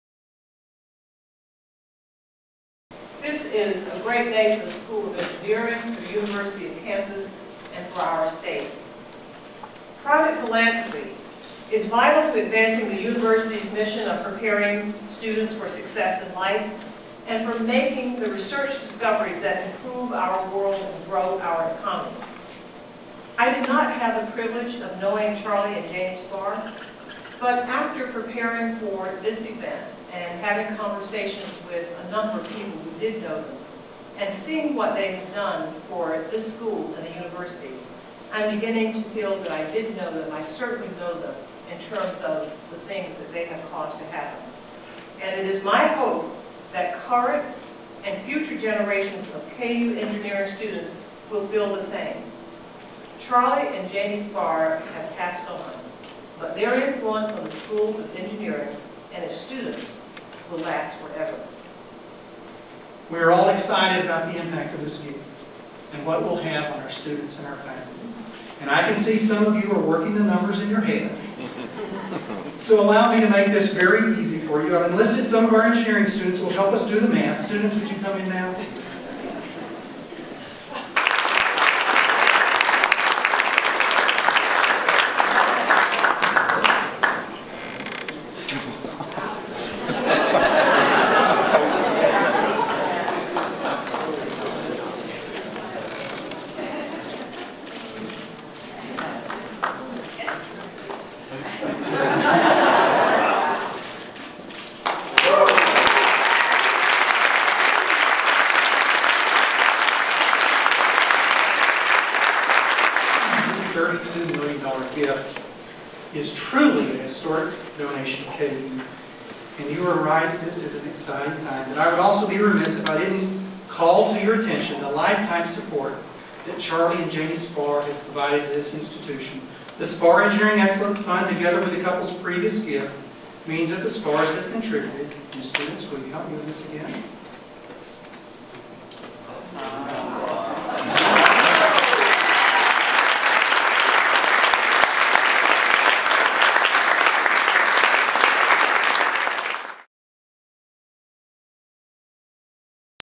Alumni leave $32 million estate gift
An estate gift of $32 million — after giving $13 million during their lifetimes — from the late Charles and Mary Jane Spahr to benefit the KU School of Engineering was announced May 13, 2011 in the Spahr Engineering Classroom. Chancellor Bernadette Gray-Little and Dean Stuart Bell spoke at the event, and students announced the large gift amount with a little help from Baby Jay.